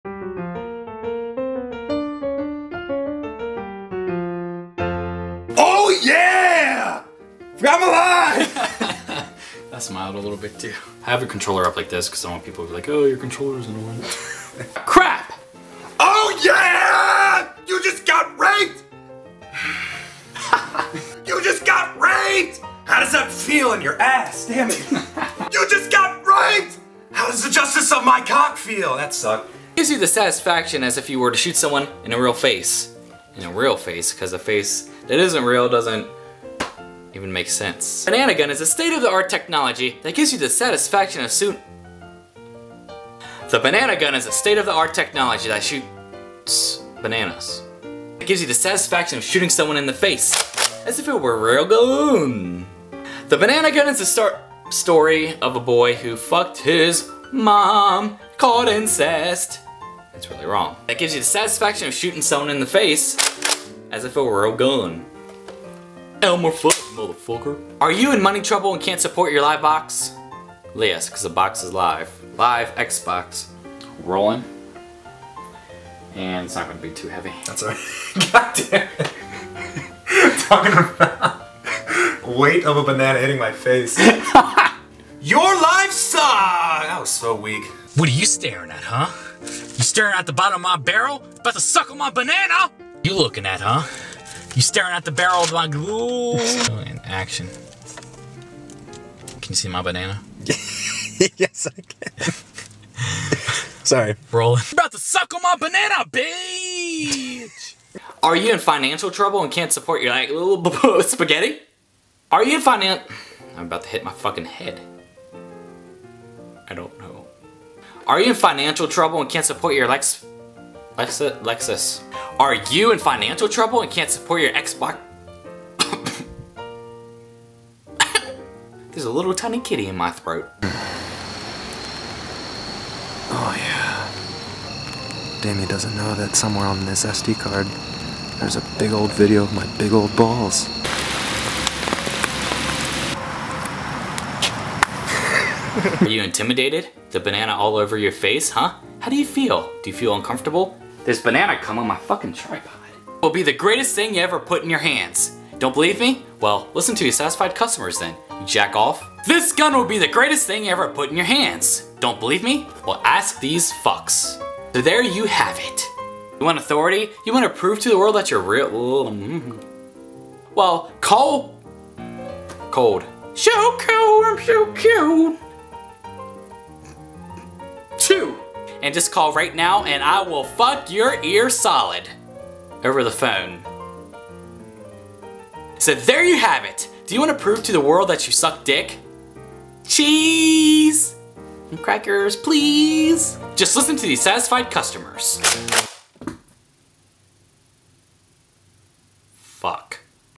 Banana Gun Bloopers!
Here are the bloopers from my video I put out yesterday, The Banana Gun! These ones are pretty good haha.